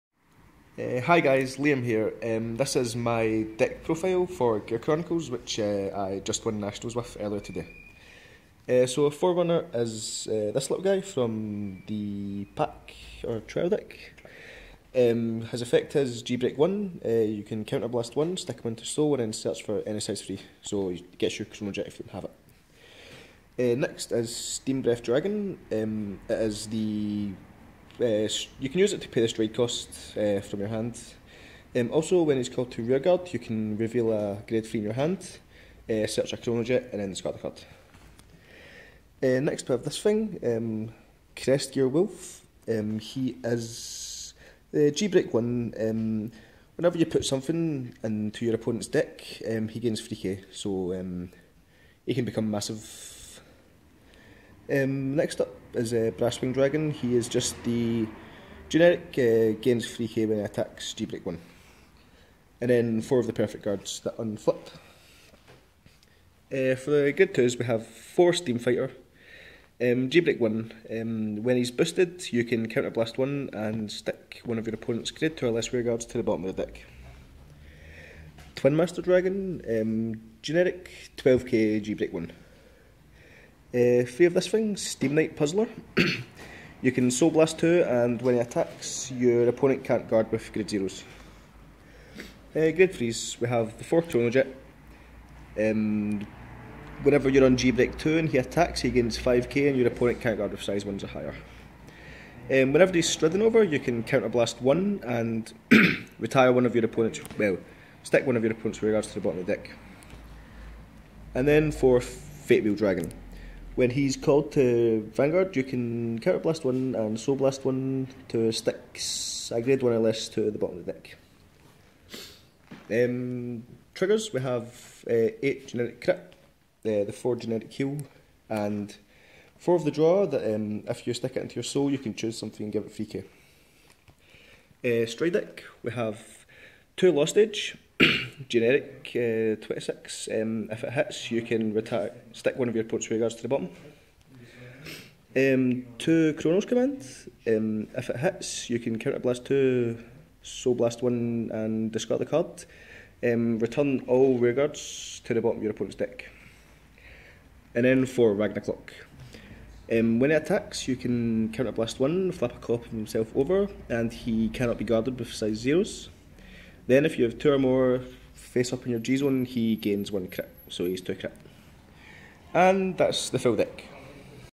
2015 National Winning Deck Gear Chronicle, Cardfight Scotland Nationals
Deck profile of KC4's Liam Haggarty, Gear Chronicle.

KC4 Winning team of the 2015 Cardfight Scotland National Team League.